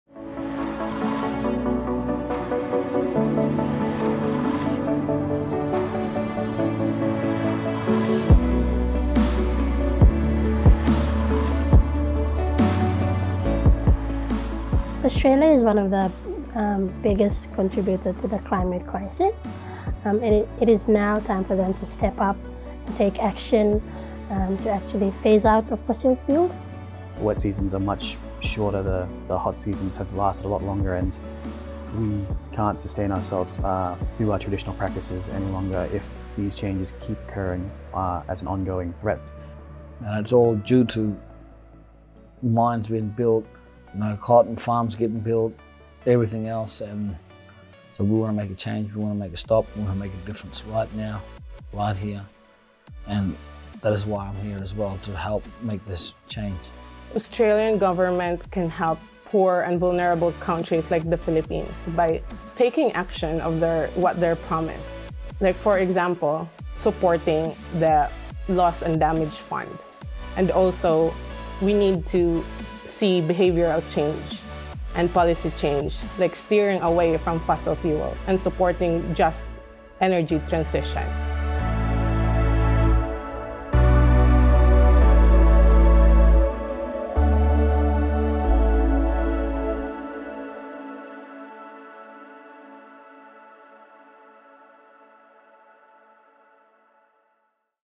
Safe Climate, Equal Future Speakers Tour
In September 2024, Oxfam Australia partnered with Seed Mob, Australia’s Indigenous Youth Climate Network, to bring youth community leaders together on the Safe Climate, Equal Future speakers tour across the country.

All the speakers have been personally impacted by the causes and impacts of climate change and are passionate about ensuring that climate-vulnerable countries and communities are not forgotten in climate action.

They brought their message to politicians and key decision-makers, Australian climate advocates, and members of the public across events in Melbourne, Sydney and Canberra. Australia must immediately end all new fossil fuel projects, and create a plan to rapidly phase out fossil fuel exports and to transition to renewable energy. Those who have benefited the most from the mining and burning of fossil fuels – big polluting corporations – need to pay for all the loss and damage they have caused communities.